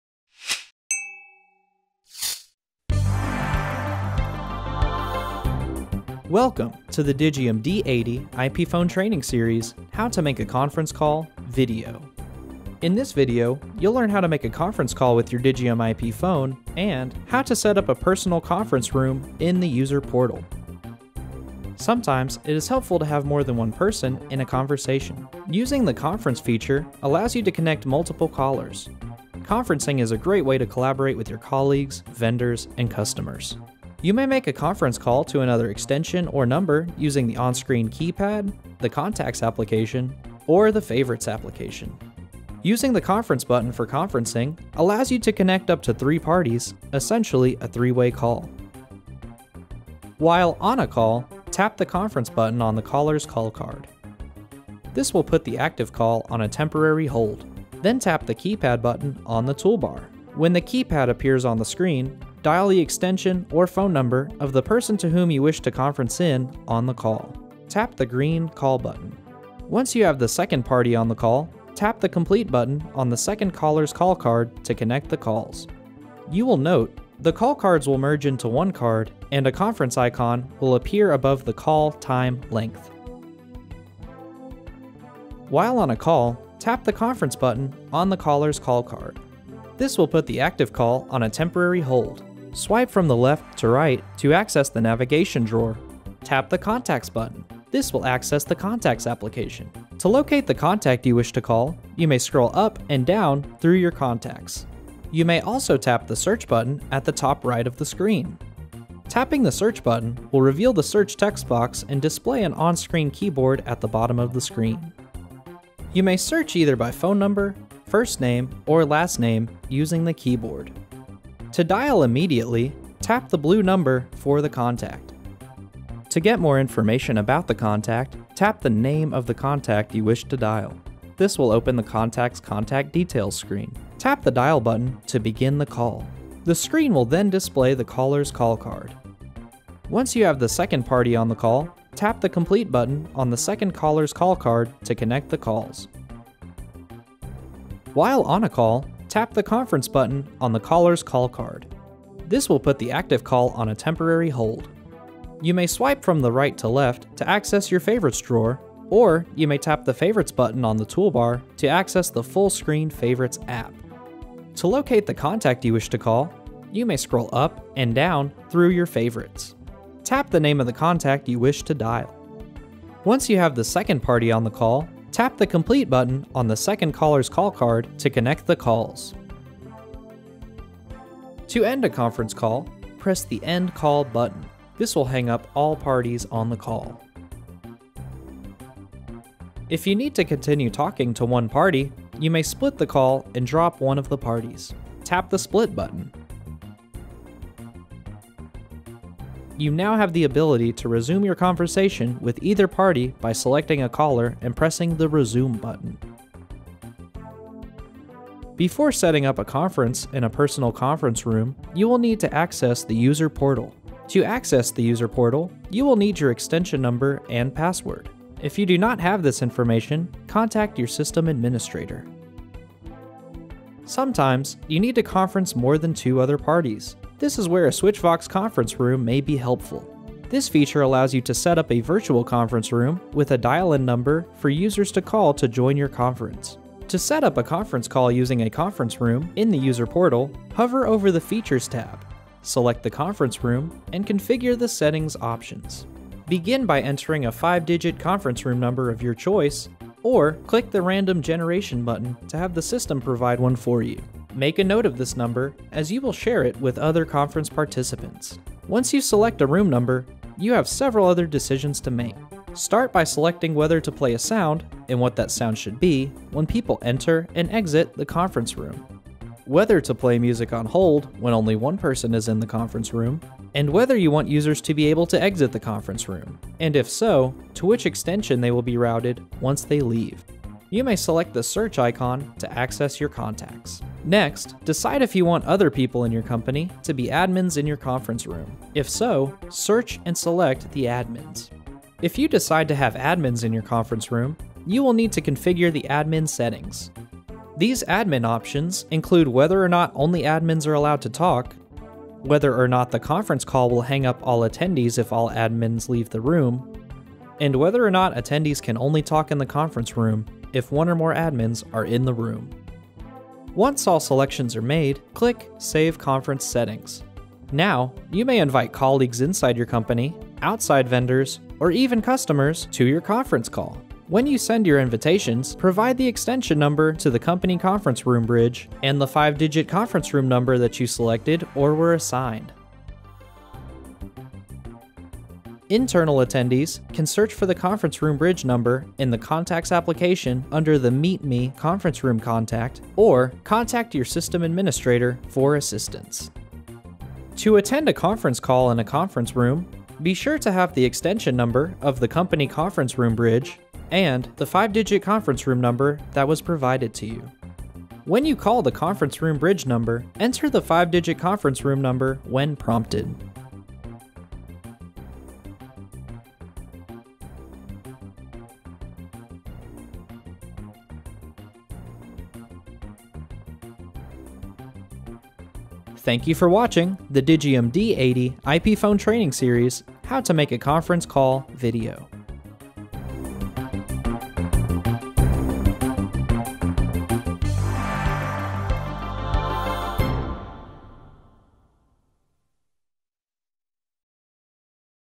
Digium D80 IP Phones Training | How to Make a Conference Call
Welcome to the Digium D80 IP Phone Training series “How to Make a Conference Call”  video.

In this video, you’ll learn how to make a conference call with your Digium IP Phone, and how to setup a personal conference room in the User Portal.

The Digium D80 series IP phones are VoIP business phones designed specifically for Asterisk based phone systems.

The Digium D80 Ip Phone features a large 7-inch HD display with multi-point touch and touch-optimized applications for call logs, visual voicemail, favorites and contacts.